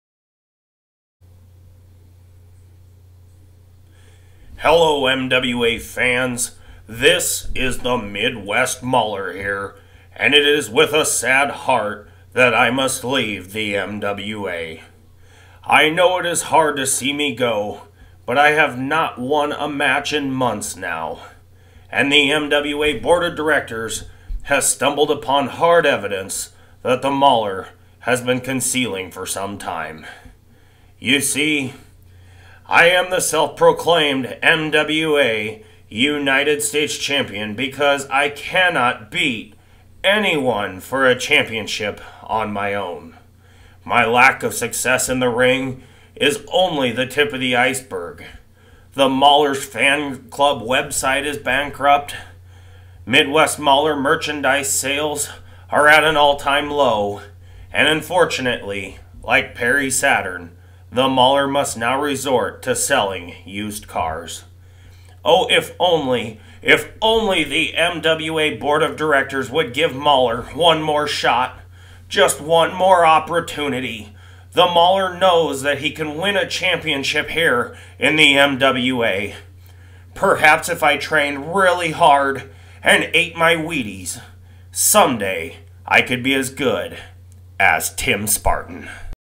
Mauler Harrassment Stage 1.avi
MWA Professional Wrestling's Midwest Mauler JB Pain says farewell.